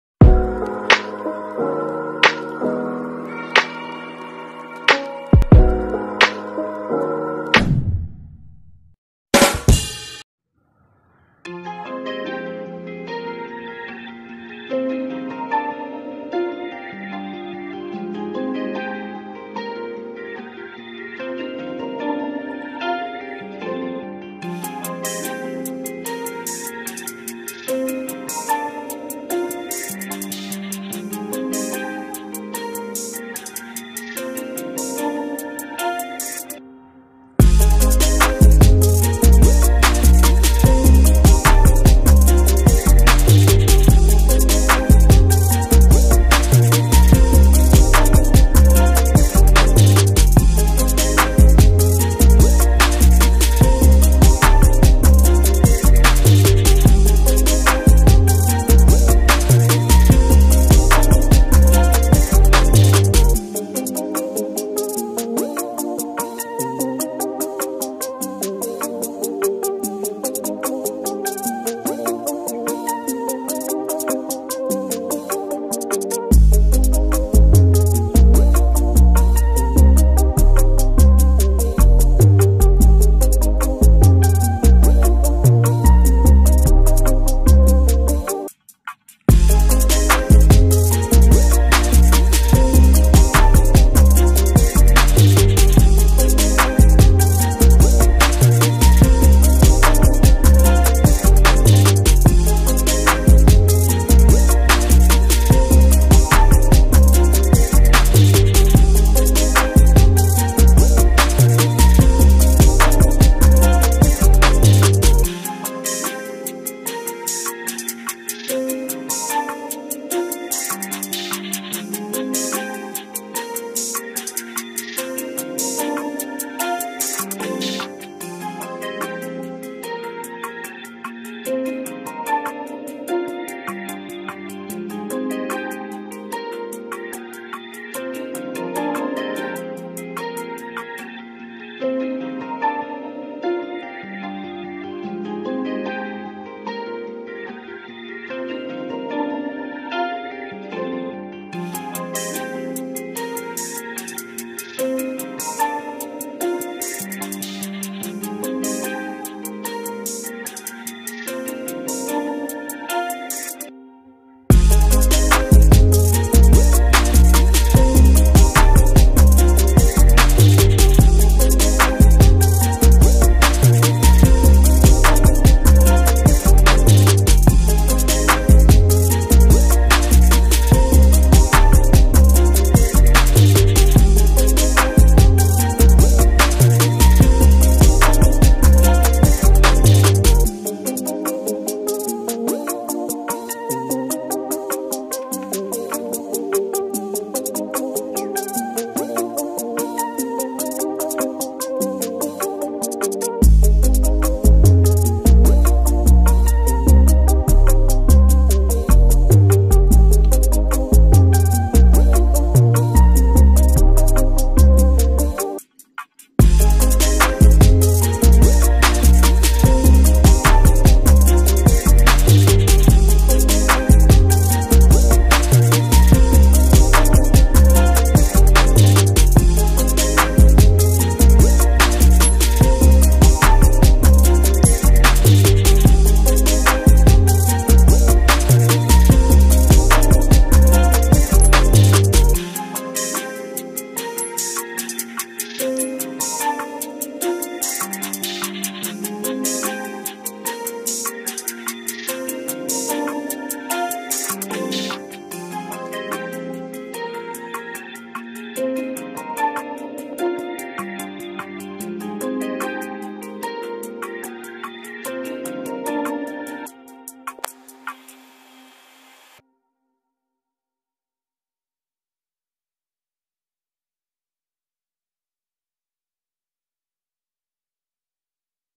Soo Beautiful Purple and Flowers in house with my Spider Man
Many of us love Flowers inside our house for beautification and refreshing and our mind if we have flowers inside our house.